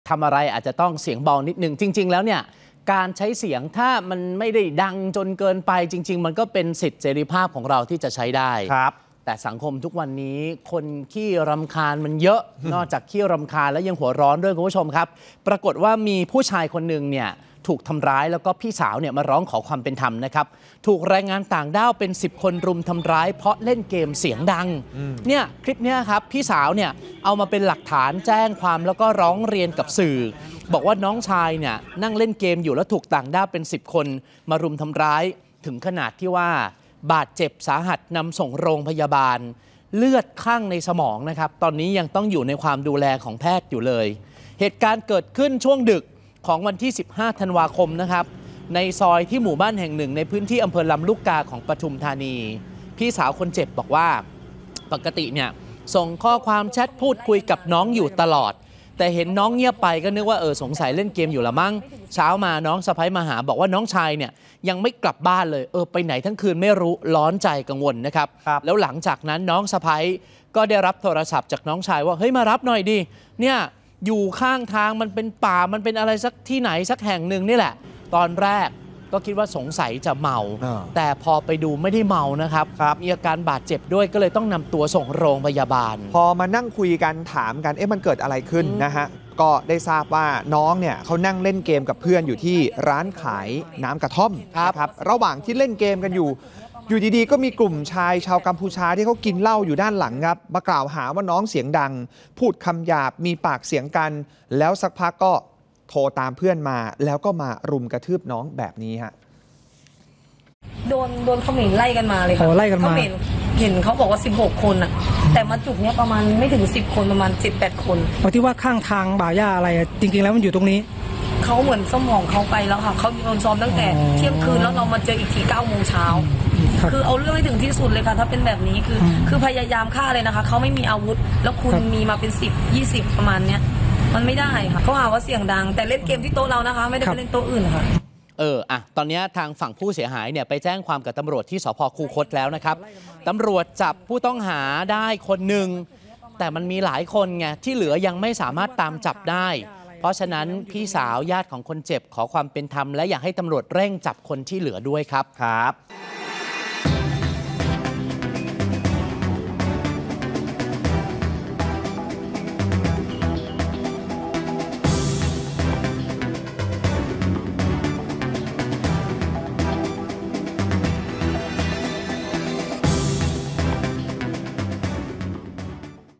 เล่นเกมเสียงดัง ถูกรุมสาหัสเลือดคั่งสมอง | ข่าวช่องวันเสาร์อาทิตย์ | สำนักข่าววันนิวส์
พี่สาวร้องขอความเป็นธรรม น้องชายถูกต่างด้าว กว่า 10 คน รุมทำร้ายร่างกาย ขณะนั่งเล่นเกม จนเลือดคั่งในสมอง